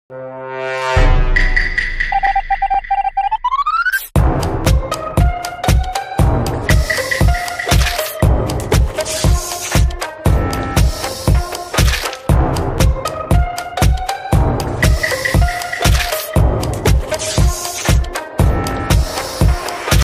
AI Drone art Stable diffusion
Art made with kaiber.ai and a drone photo